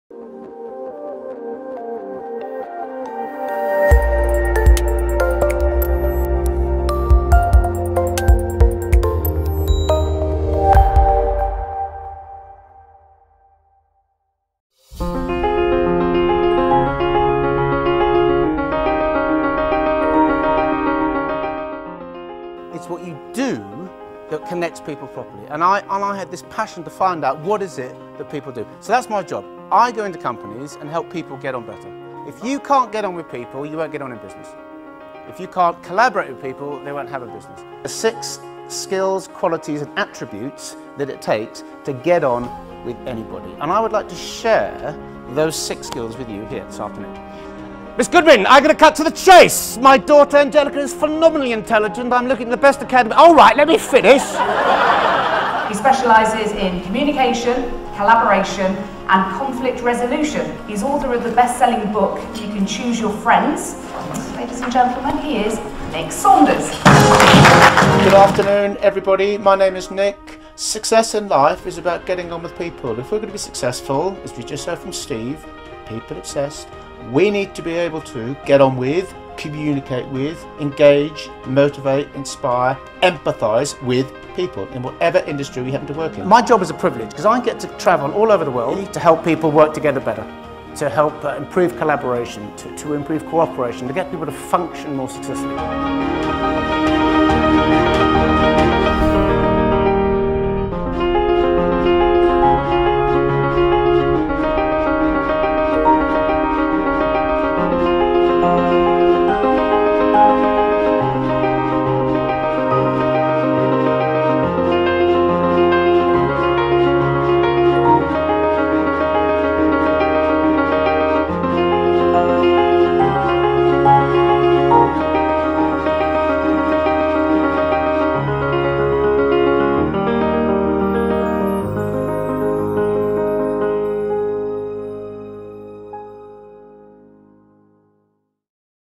Nick Saunders - Communication Speaker
Nick Saunders is a keynote speaker, trainer and author on the topic of communication and collaboration.

To book Nick to speak at your event, please visit his profile

His keynote speeches help individuals and teams work together more effectively, communicate and collaborate more successfully.

Nick is the author of the best-selling book You Can Choose Your Friends: The 6 Key Skills That Turn Business Relationships Into Results.

Often referred to as The Family Man, Nick demonstrates that workplaces are just like families; you can choose your friends, but work colleagues and families you’re stuck with.